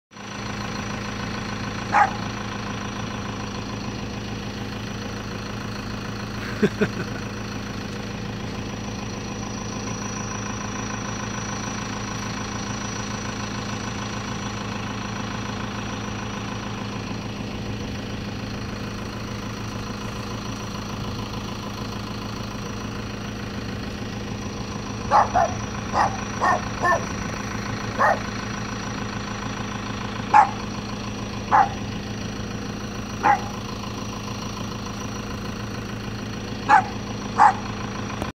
My "My Little Pony" moment
2 week old miniature horse with it's dame.